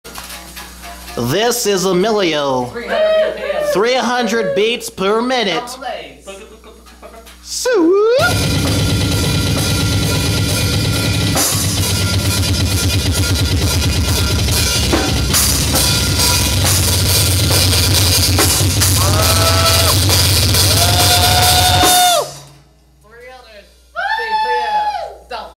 300 bpm double bass
Just messing around having fun with my band after practice.